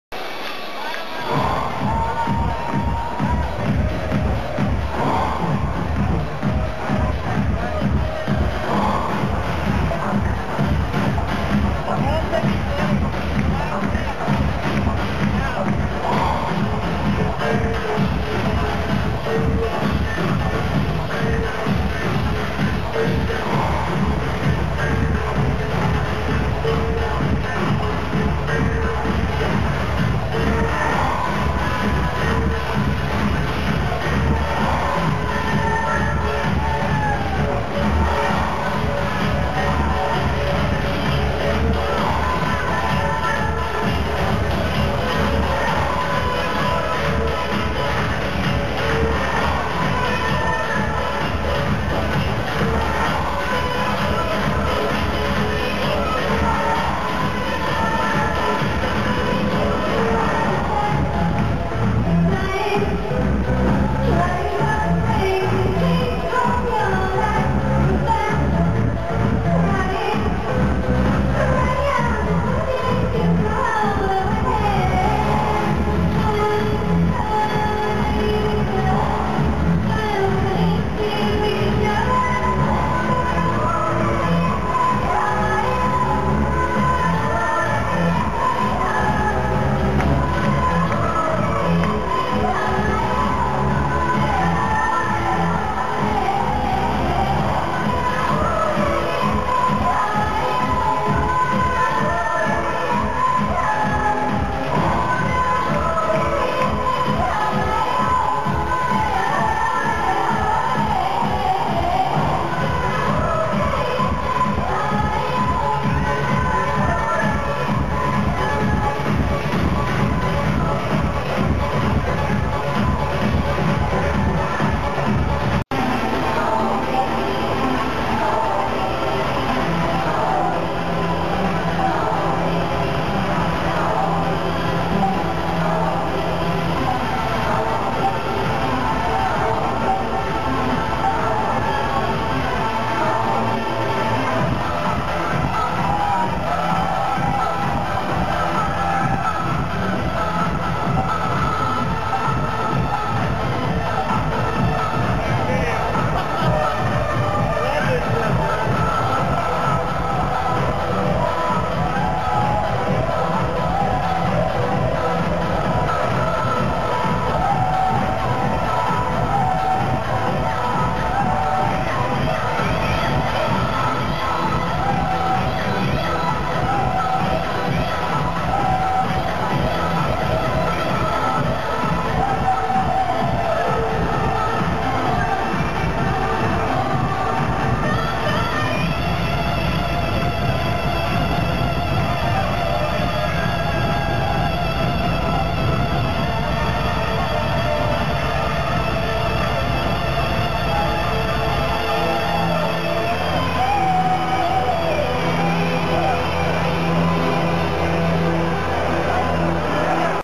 DJ James Andersen Spinning at the Last Dance for Roxy NYC
This is a conbination of different videos when DJ James Andersen started spinning at the Last Dance of Roxy NYC 03/10/07.